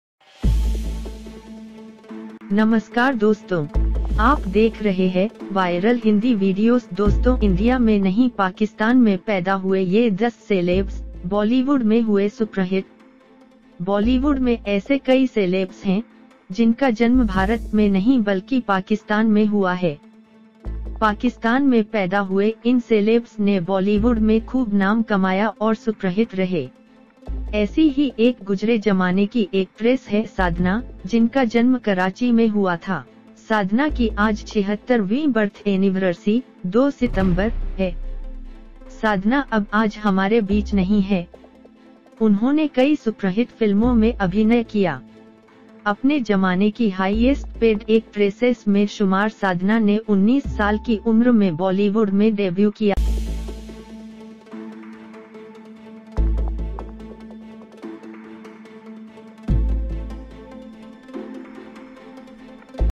इंडिया में नहीं पाकिस्तान में पैदा हुए ये 10 सेलेब्स, बॉलीवुड में हुए सुपरहिट
बॉलीवुड में ऐसे कई सेलेब्स हैं, जिनका जन्म भारत में नहीं बल्कि पाकिस्तान में हुआ है। पाकिस्तान में पैदा हुए इन सेलेब्स ने बॉलीवुड में खूब नाम कमाया और सुपरहिट रहे। ऐसी ही एक गुजरे जमाने की एक्ट्रेस है साधना, जिनका जन्म कराची में हुआ था। साधना की आज 76वीं बर्थ एनिवरर्सी (2 सितंबर) है। साधना अब आज हमारे बीच नहीं है। उन्होंने कई सुपरहिट फिल्मों में अभिनय किया। अपने जमाने की हाइएस्ट पेड एक्ट्रेसेस में शुमार साधना ने 19 साल की उम्र में बॉलीवुड में डेब्यू किया था।